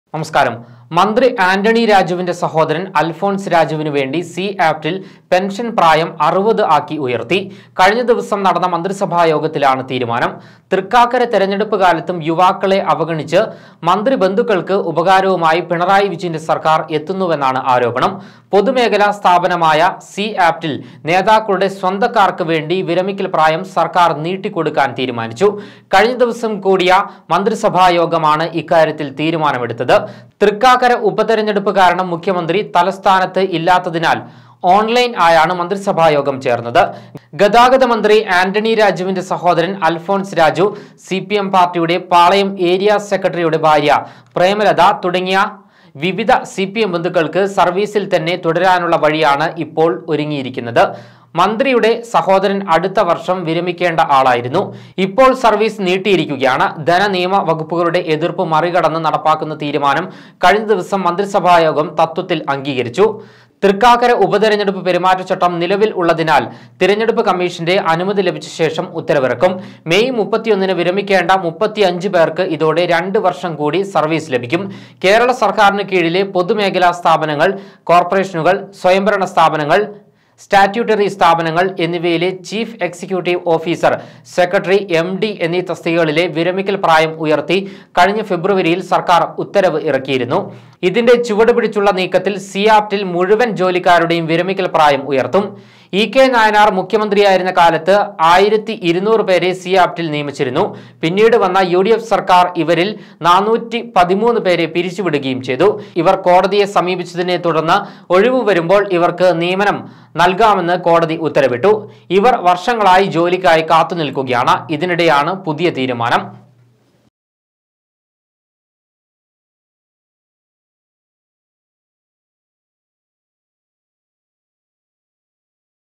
ആന്റണി രാജുവിന്റെ സഹോദരന് വേണ്ടി പെൻഷൻ പ്രായം ഉയർത്തി? I pension age
തൃക്കാക്കര തിരഞ്ഞെടുപ്പ് കാലത്തും യുവാക്കളെ അവഗണിച്ച് മന്ത്രിബന്ധുക്കൾക്ക് പാരിതോഷികം; സി-ആപ്റ്റിൽ പെൻഷൻ പ്രായം 60 ആക്കി ഉയർത്തിയത് മന്ത്രി ആന്റണി രാജുവിന്റെ സഹോദരന് വേണ്ടിയെന്ന് ആരോപണം; അൽഫോൻസ് രാജുവിന് പുറമേ മറ്റു സിപിഎം ബന്ധുക്കൾക്കും സർവീസിൽ തുടരാം; തീരുമാനം കഴിഞ്ഞ മന്ത്രിസഭാ യോഗത്തിൽ